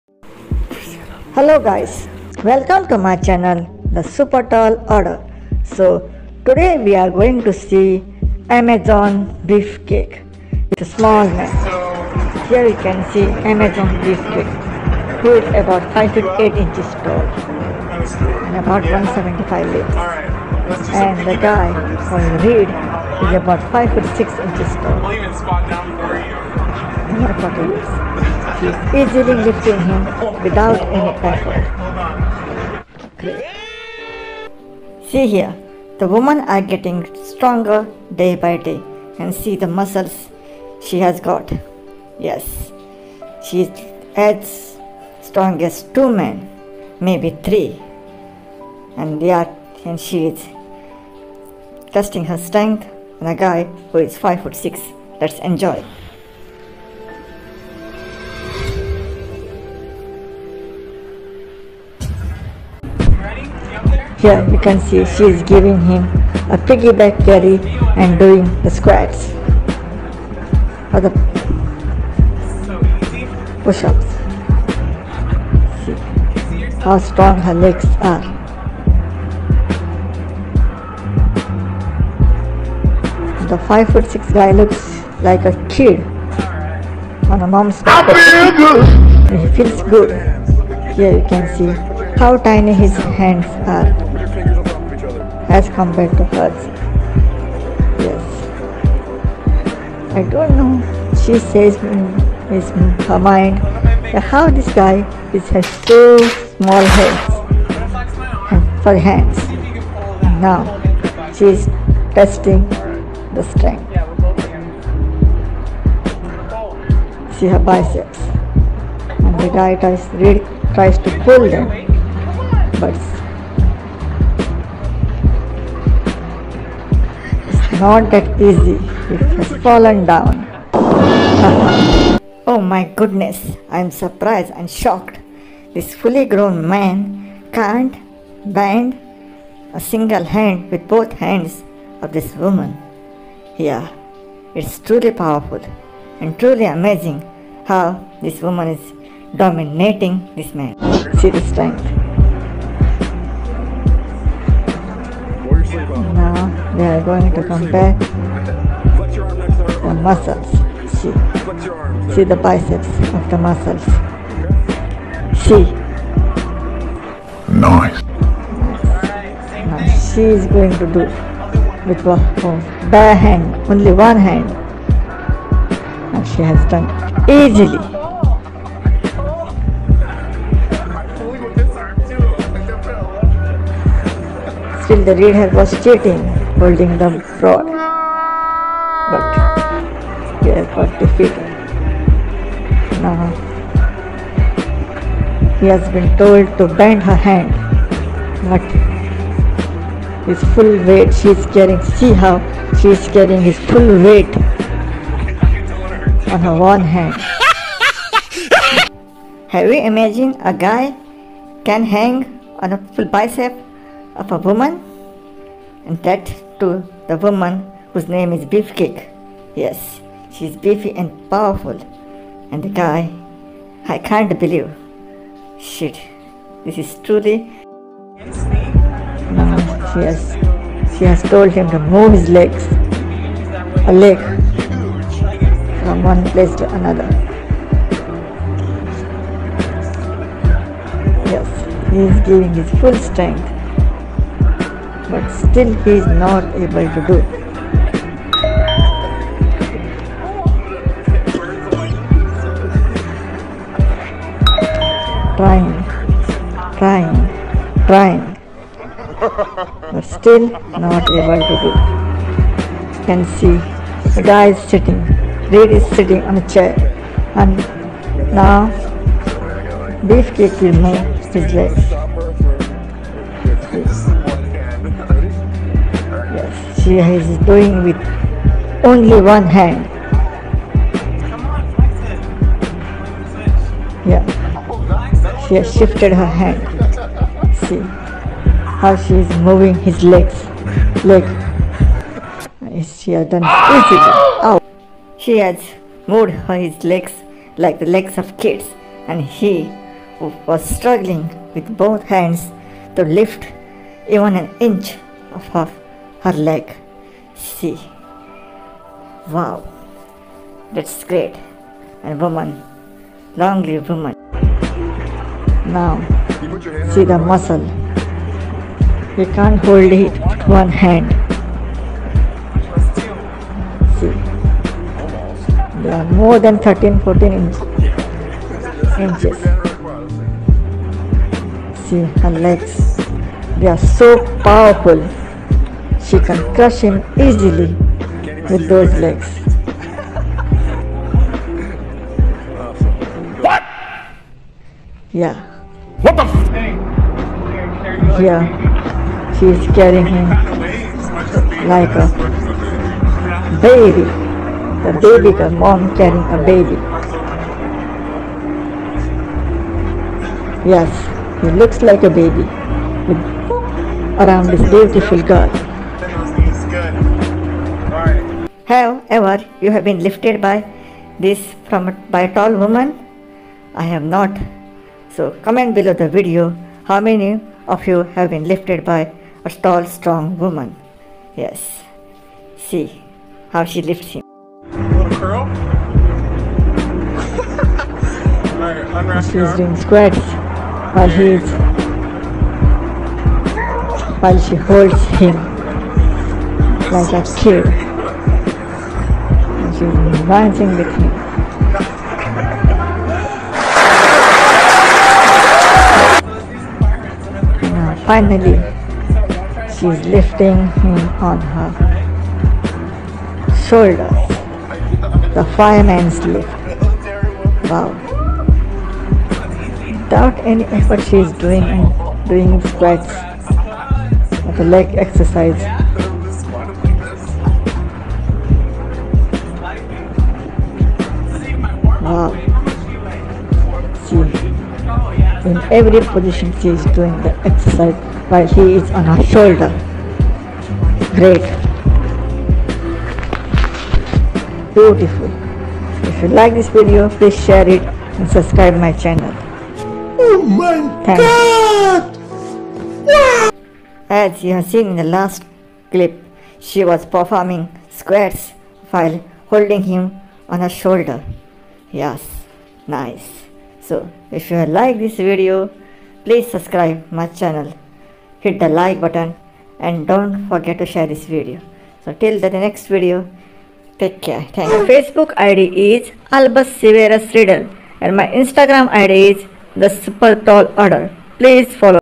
Meet the Amazon - 3 | Amazon Beef Cake | tall amazon woman
jai siyaram

A tall muscular and beautiful Amazon Beef Cake compares herself with 5 feet 6 guy. She is tall and muscular. She is 5 feet 9 inches without heels.